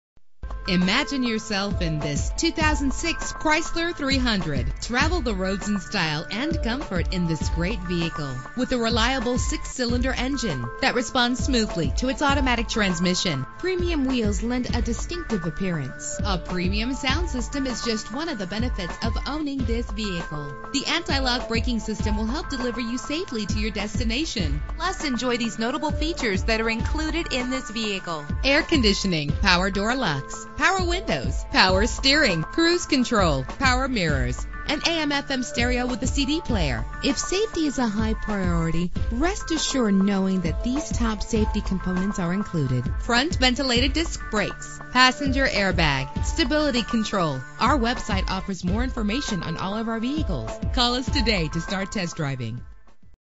2006 Chrysler 300 - Irvine CA
Year: 2006
Make: Chrysler
Model: 300
Trim: Touring
Engine: 3.5 liter 6 cylinder 24 valve
Transmission: Automatic
Color: White
Mileage: 59418
Address:
16 Auto Center Dr